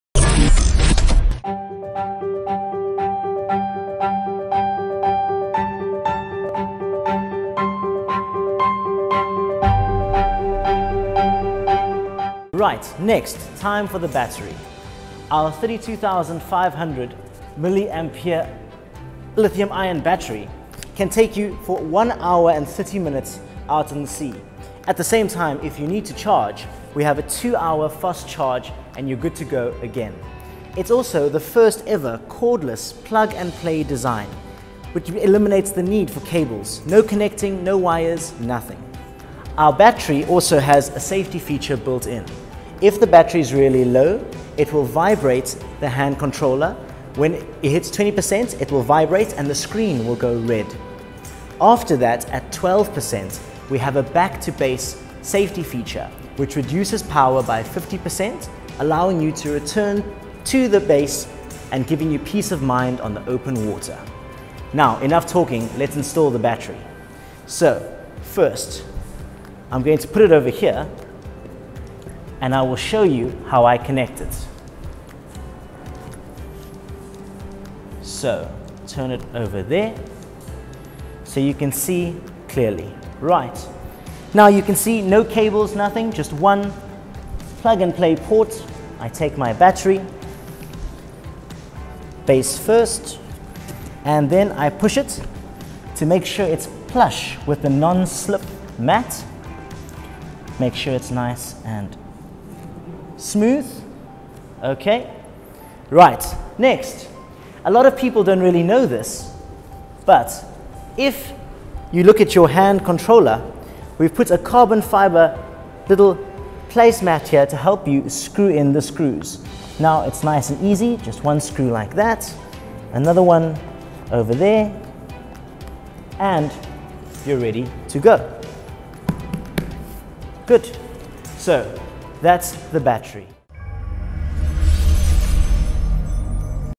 Waydoo Flyer ONE eFoil Tutorial - PowerFlight Cell Battery Intro and Installation
Official video demonstration of how to set up the Waydoo Flyer ONE eFoil.